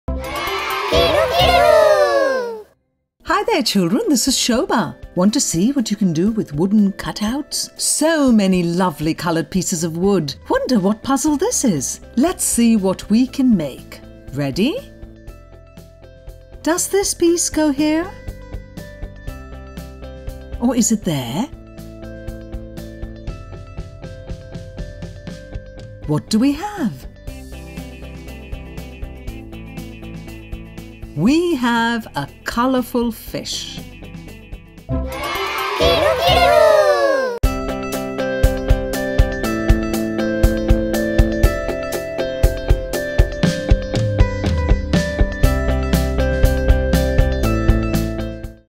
Wooden Puzzles for Preschoolers - Colorful Fish Interactive Learning Video
Kids we have another puzzle for you to make at home today and its an eye catching multicolored fish. This interactive learning video will help kids and adults assemble this fish at home and enjoy a great family experience. No age restrictions and no limits to the fun!